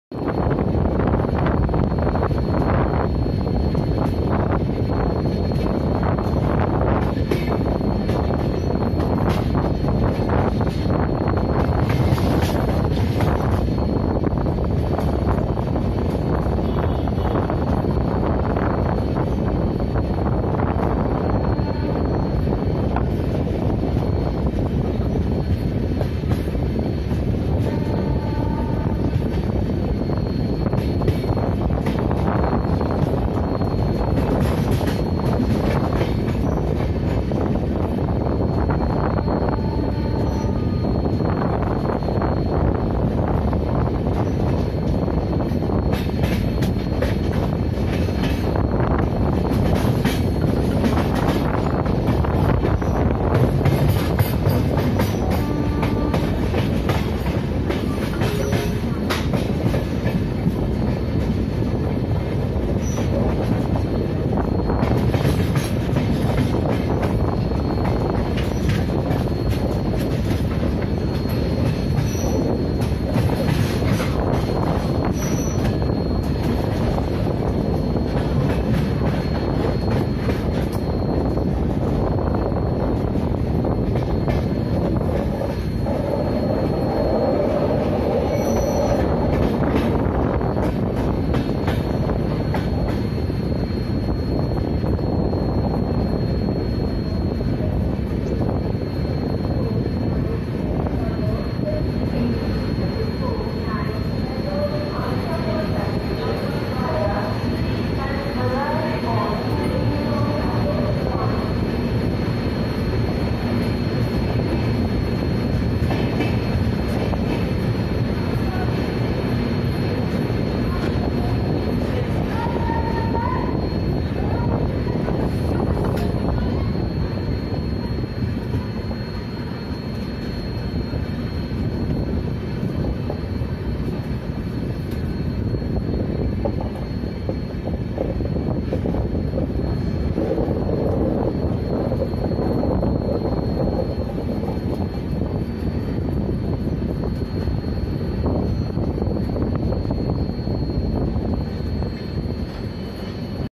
Santragachi Habibganj Humsafar Skips Anuppur Jn
22170 SRC HBJ HUMSAFAR SCHEDULE SKIP OF ANUPPUR JN